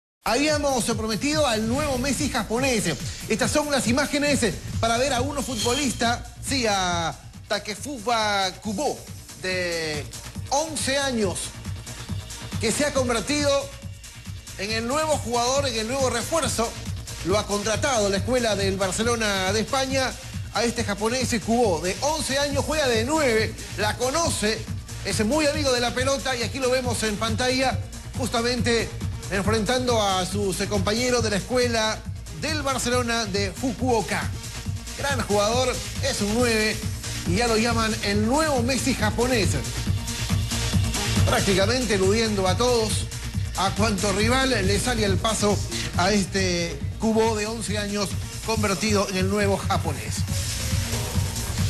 GLOBAL NOTICIAS - El "Messi" japonés.
Pequeño japonés de nueve años juega al fútbol de una manera alucinante.